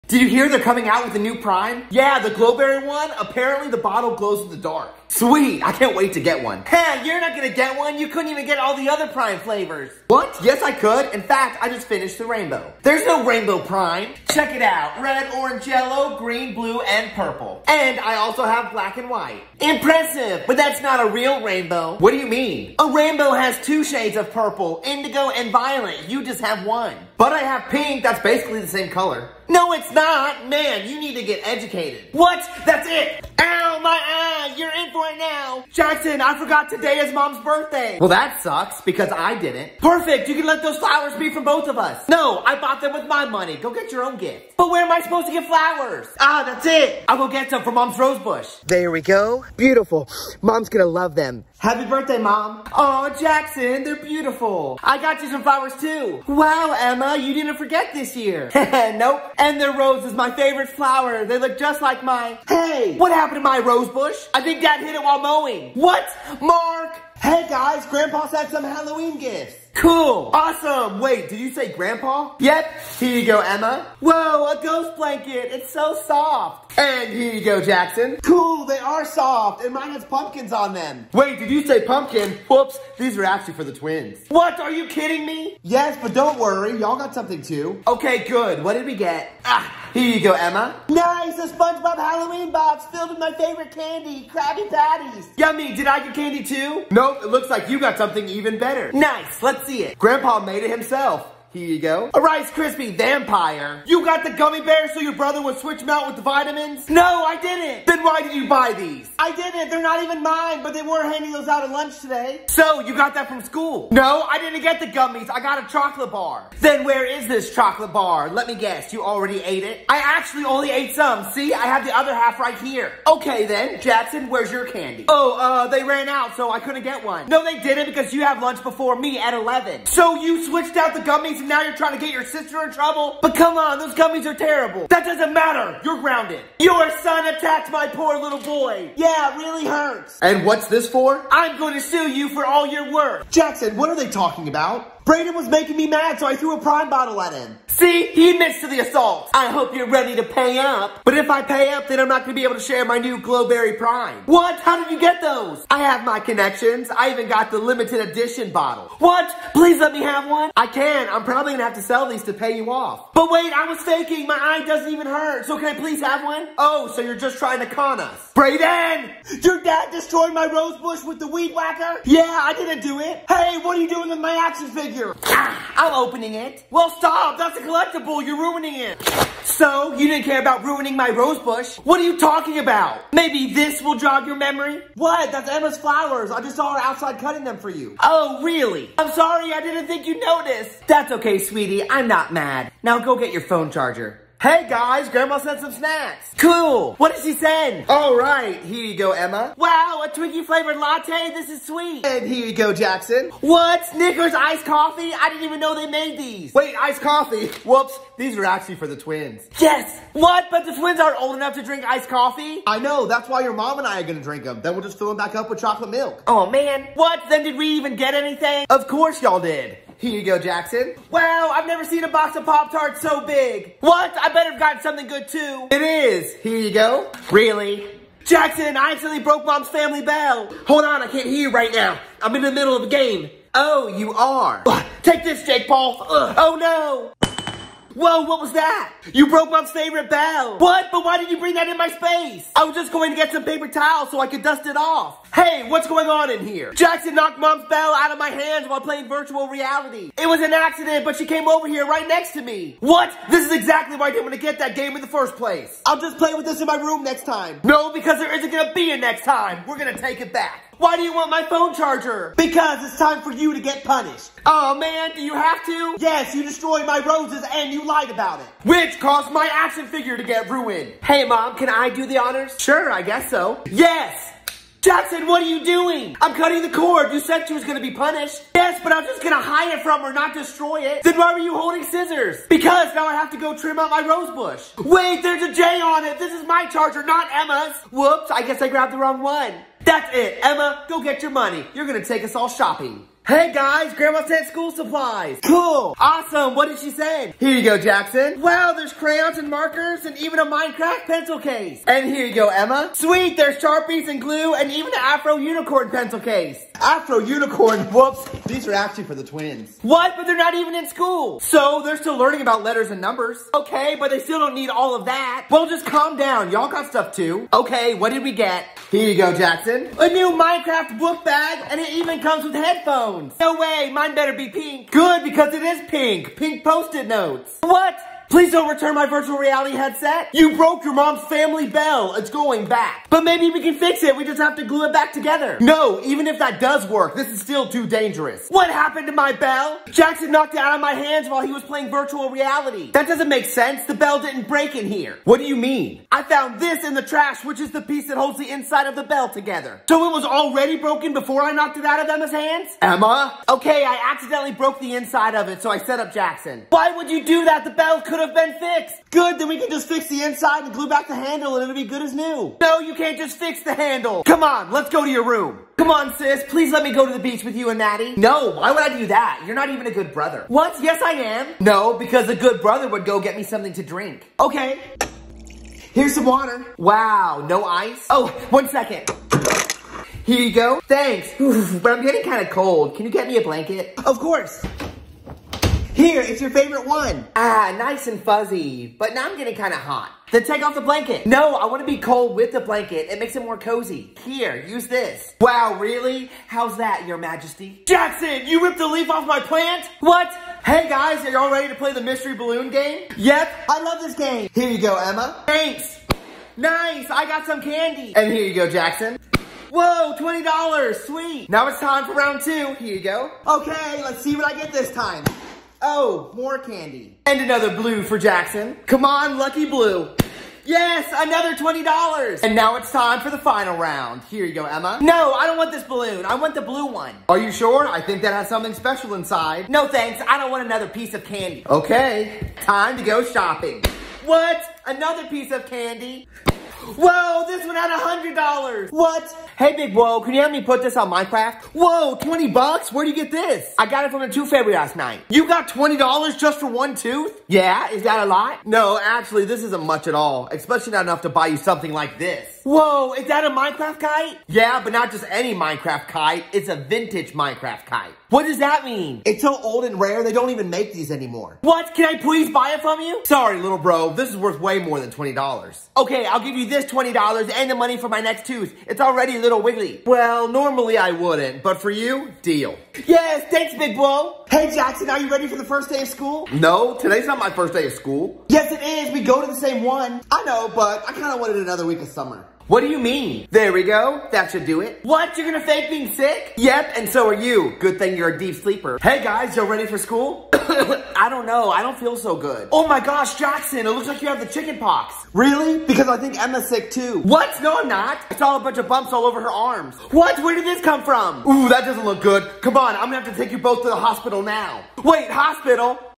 New 2023 Funny Mark Ryan Viral TikTok Compilation Part 6!!!
Watch the newest viral Mark Ryan TikToks with Jackson and Emma!